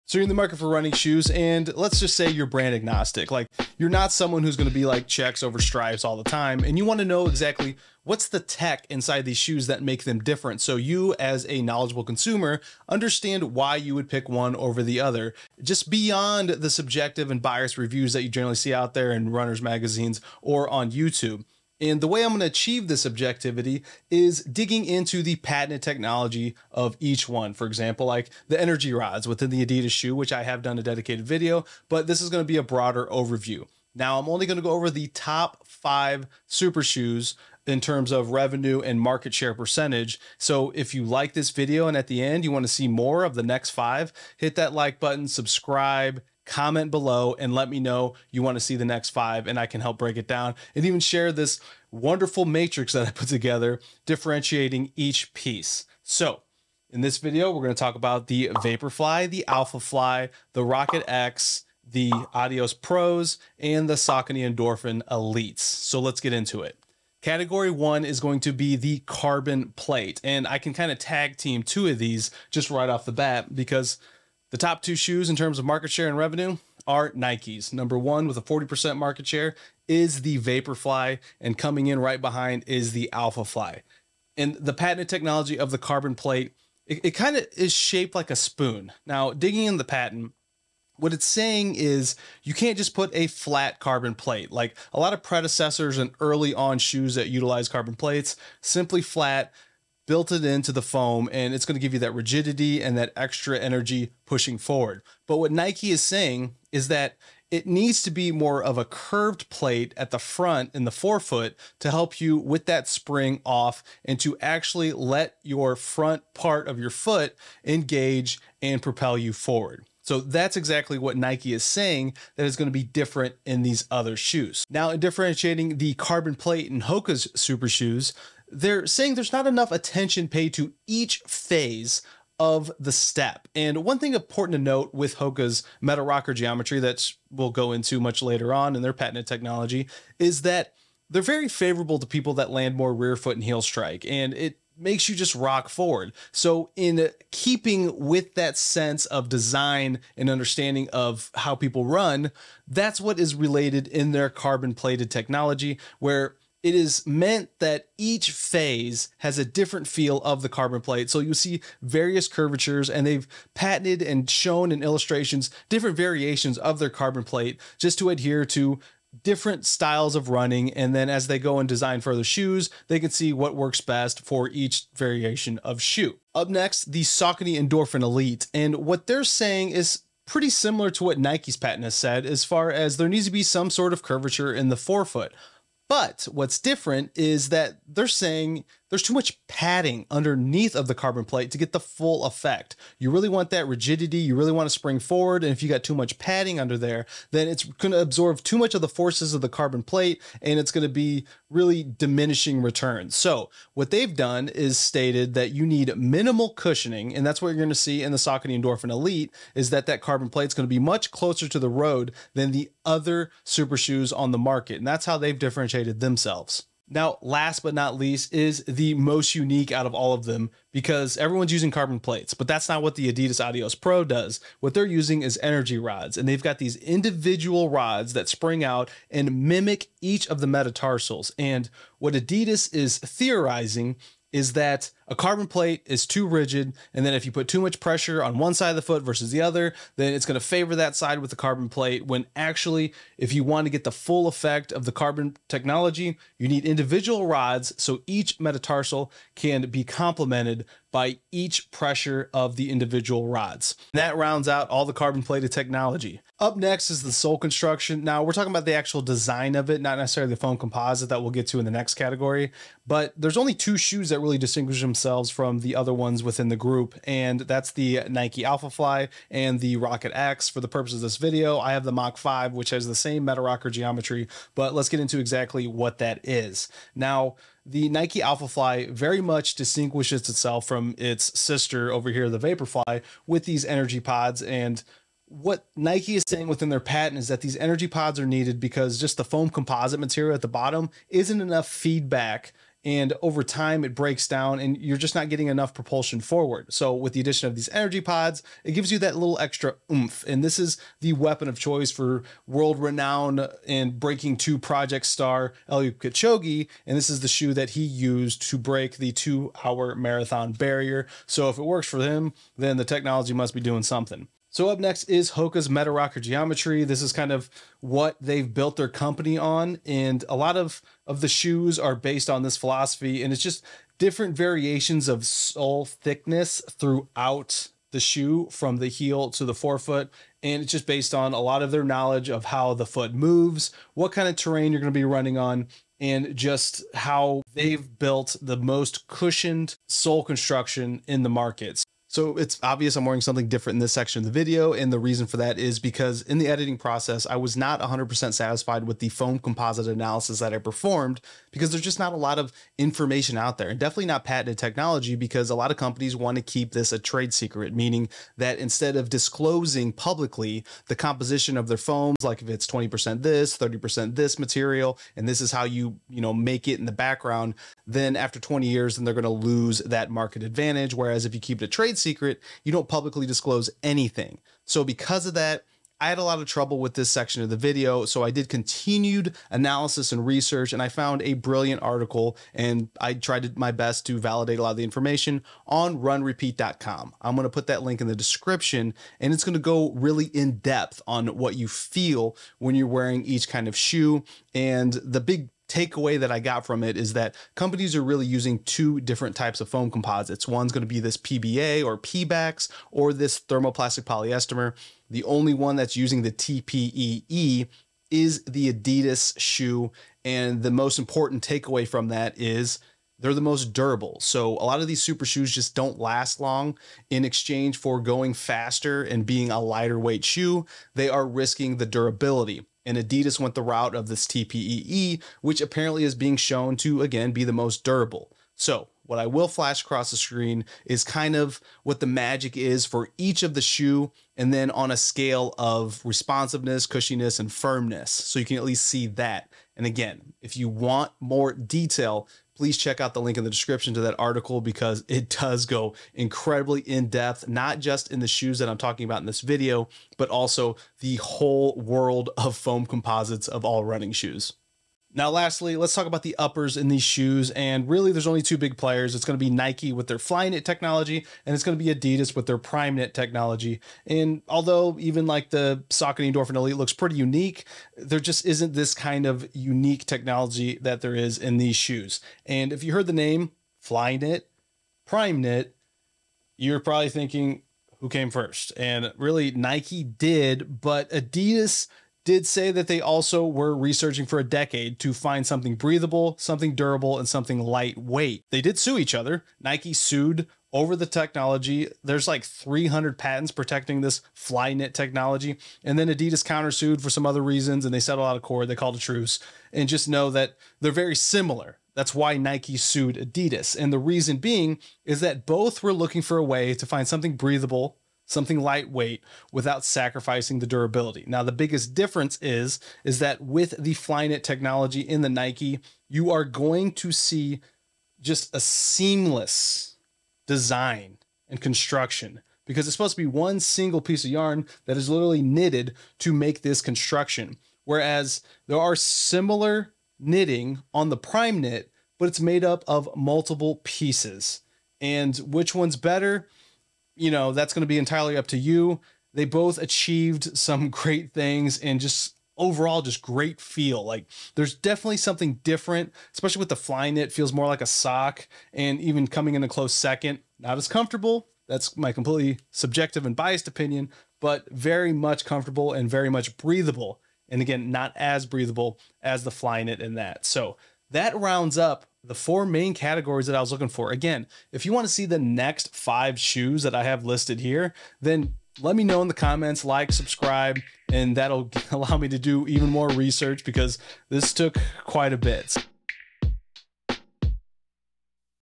ACTUAL TECHNICAL DIFFERENCE of the Top 5 Running Shoes
In this video, I breakdown the top 5 selling carbon-plated running shoes by their patented technology. This objective comparison will hopefully help you in your buying preference.

TIMESTAMPS:
00:00 - INTRO
01:06 - CARBON PLATE TECH
04:16 - SOLE DESIGN
05:41 - FOAM COMPOSITE
07:35 - UPPER DESIGN

To get your own Eight Sleep mattress or pod pro cover use the following link and code 'WES' for $100 Off at checkout